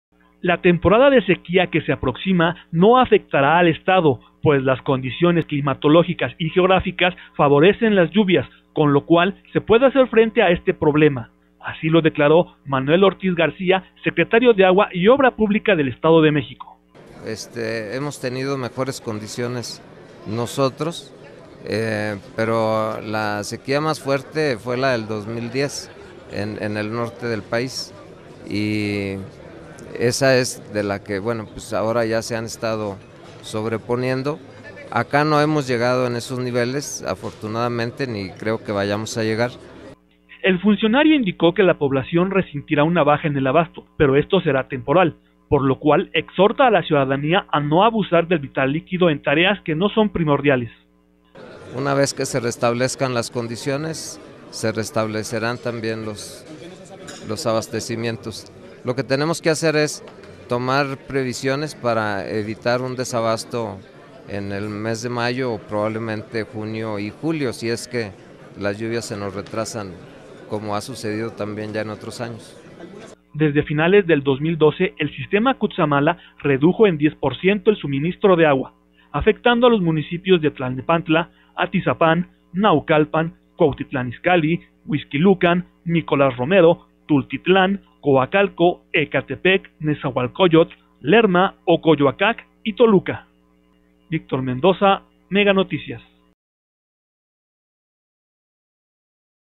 NO SE DECLARA EN EDOMEX ALERTA POR SEQUIA
El Secretario de Agua y Obra Pública del Estado de México, Manuel Ortíz García negó que en la próxima temporada de sequía se presente una alerta en la entidad.